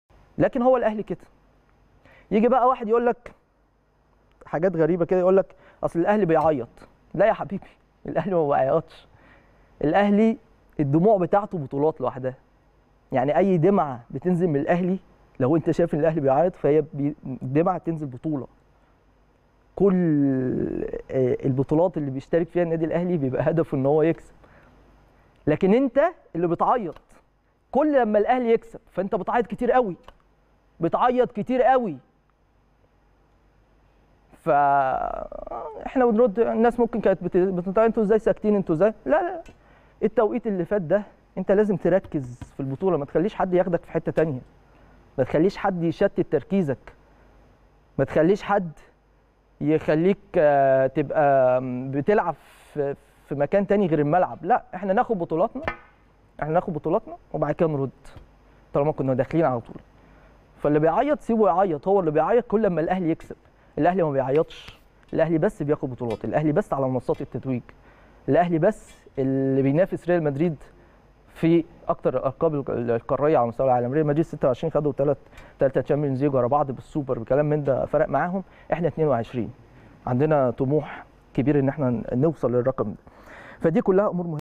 أمير هشام: "الأهلي مبيعيطش الدمعه منه ببطولة.. لكن انت بتعيط في كل بطولة للأهلي وكدة هتعيط كتير أوي"
أمير هشام: "الأهلي مبيعيطش الدمعه منه ببطولة.. لكن انت بتعيط في كل بطولة للأهلي وكدة هتعيط كتير أوي" | التريند